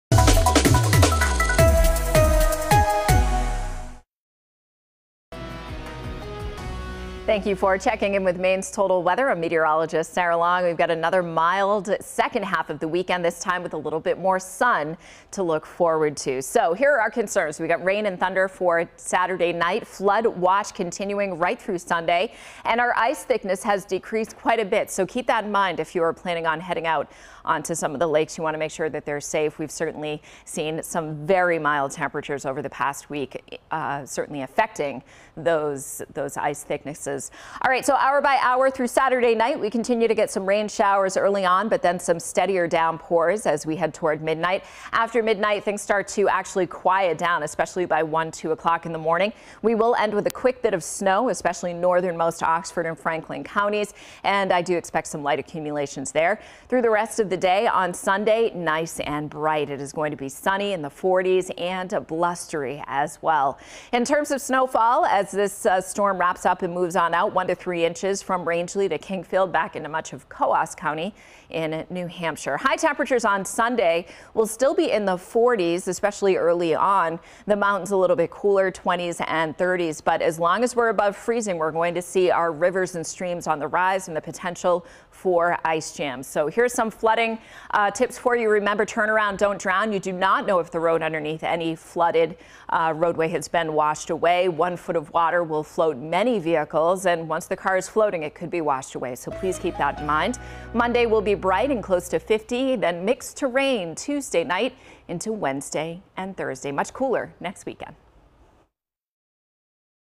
Sarah’s Saturday Evening Forecast
How’s the weather looking for your Saturday evening? Get the latest Maine’s Total Weather video forecast from Meteorologist Sarah Long.